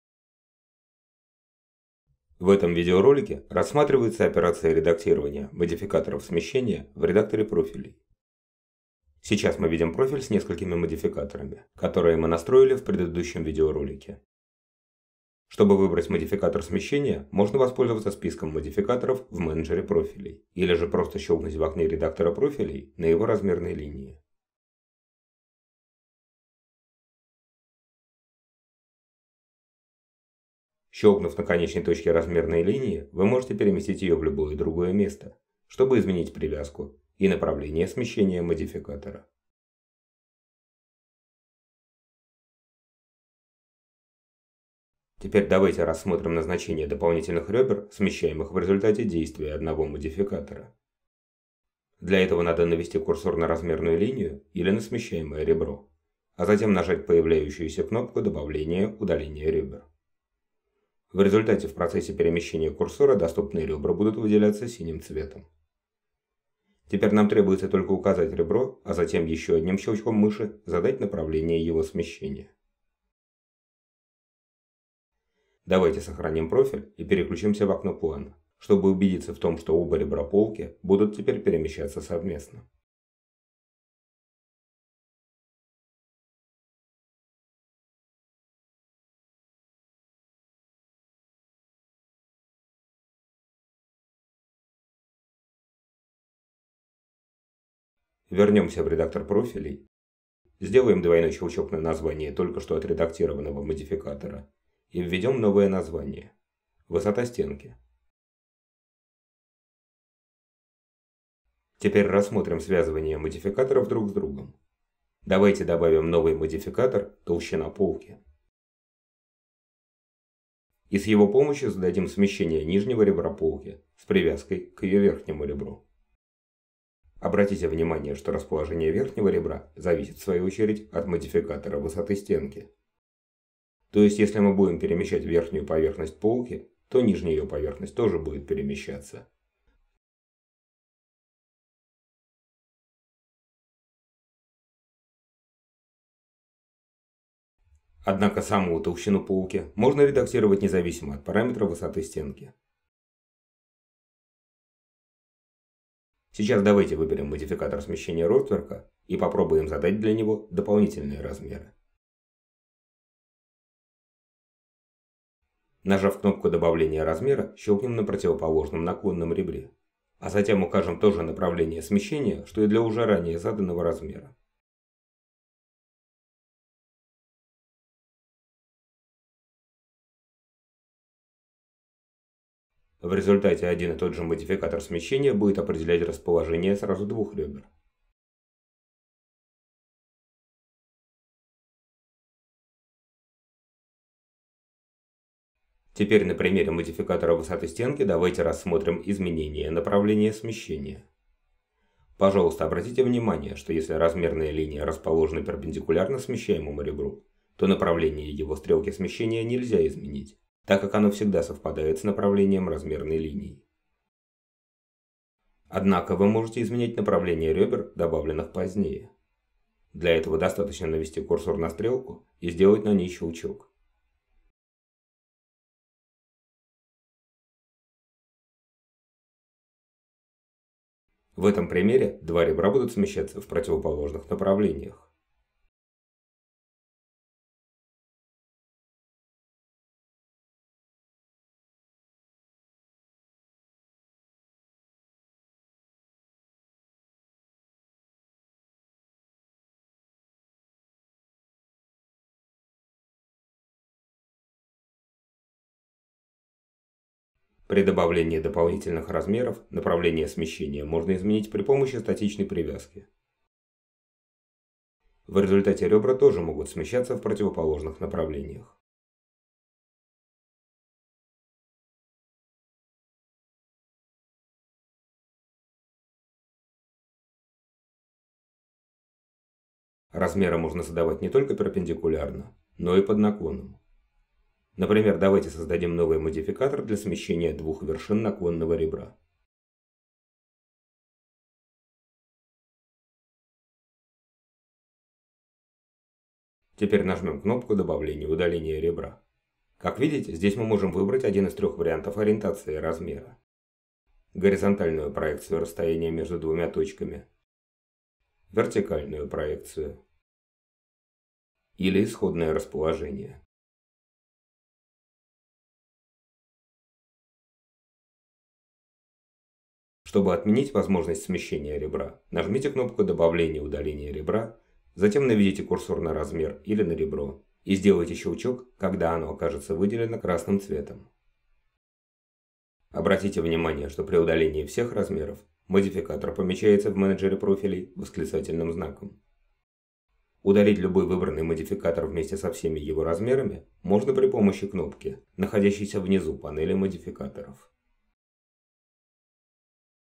Редактирование Модификаторов Смещения
В этом видеоролике рассматриваются операции редактирования Модификаторов Смещения в Редакторе Профилей.